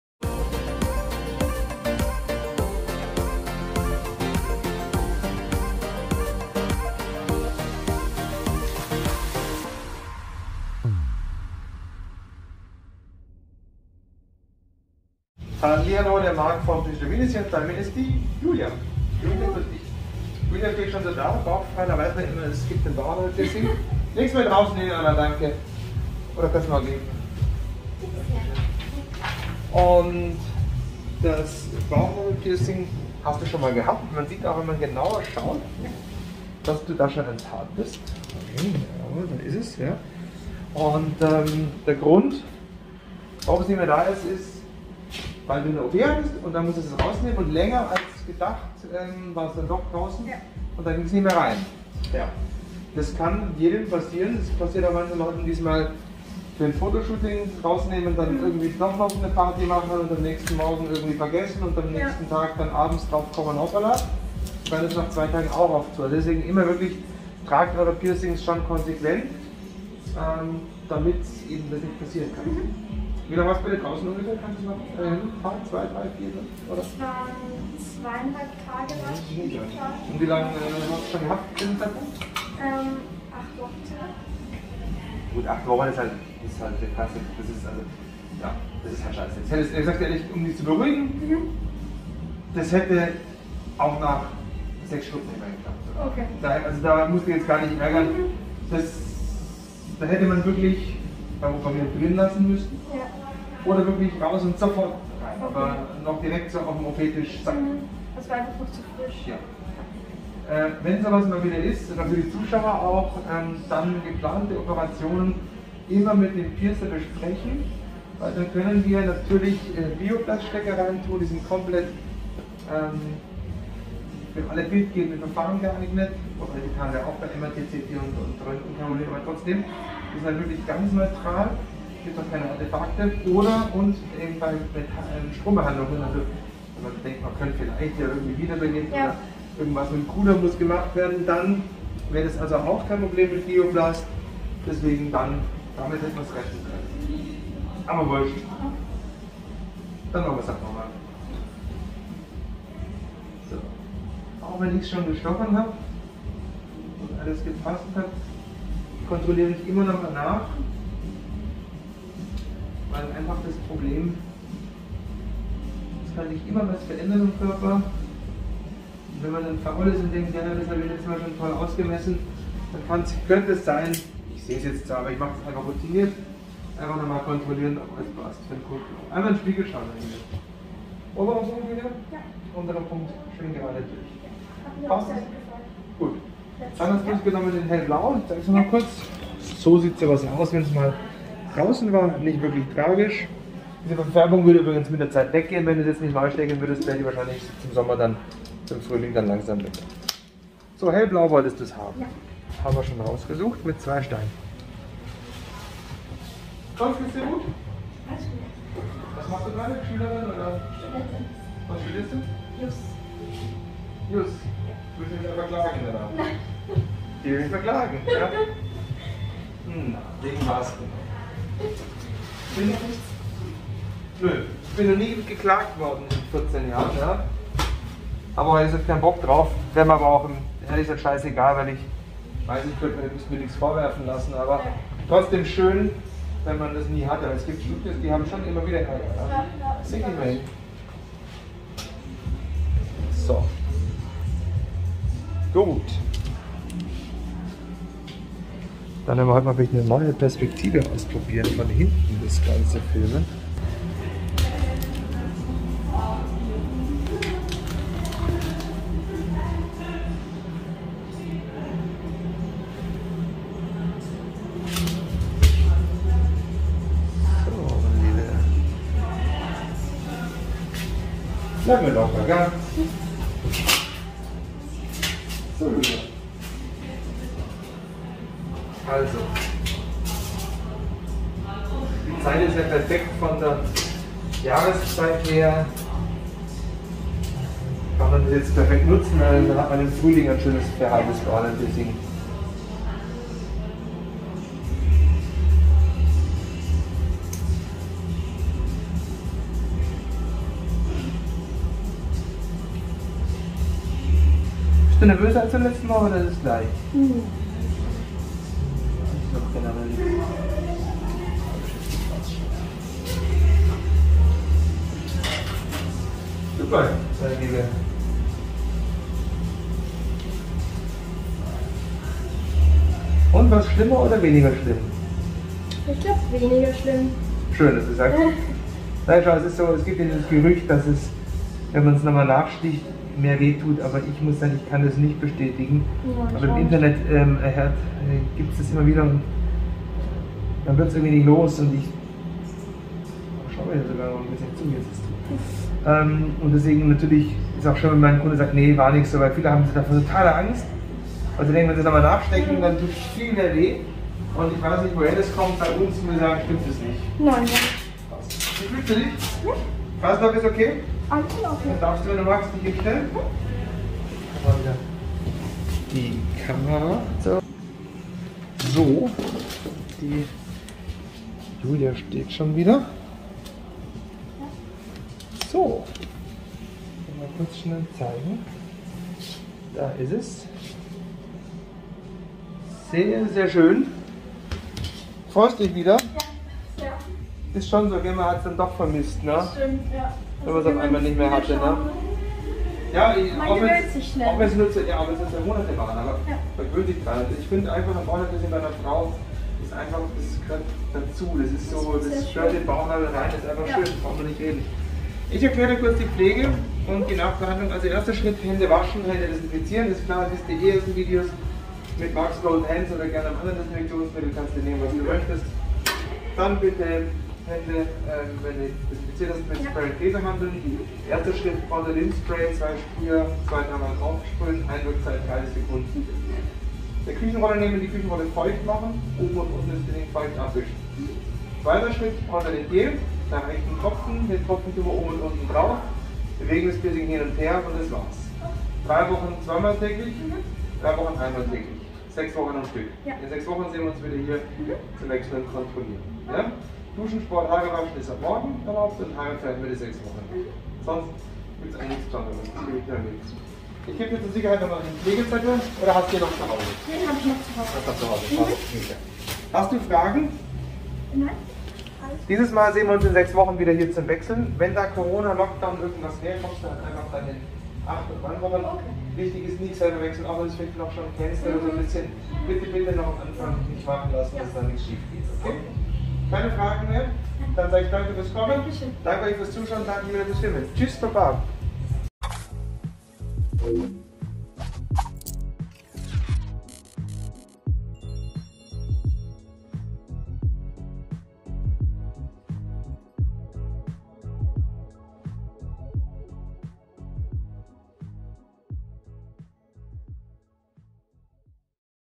OP oh jeh! Bauchnabel Piercing zugewachsen. Wir versuchen es rekonstruieren 🔥 💉 Marc's Piercing TV
Hier der Link zu Marc's Piercing Pflegeanleitung zum GRATIS Download!

Willkommen bei Marc's Piercing TV - Dem Piercing YouTube Sender live vom Piercingstudio Wien.